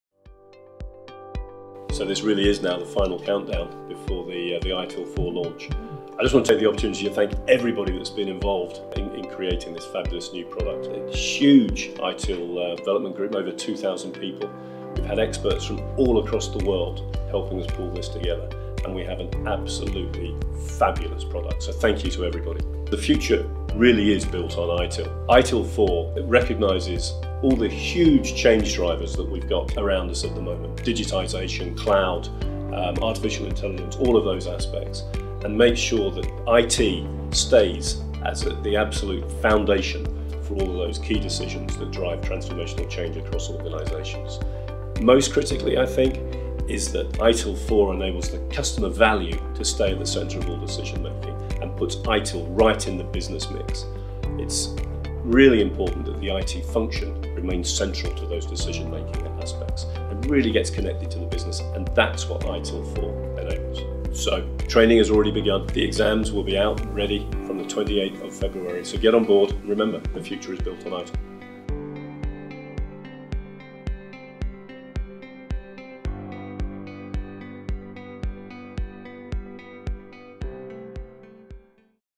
ITIL 4 – Welcome to the Future!
AXELOS CEO Mark Basham discusses how ITIL® 4 will ensure that IT stays as the foundation for all those key decisions that drive transformational change across the organization.

Still the ultimate in IT best practice, ITIL 4 builds on all the best things from ITIL and expands IT and service operations into the future.

Welcome to the Future of ITIL!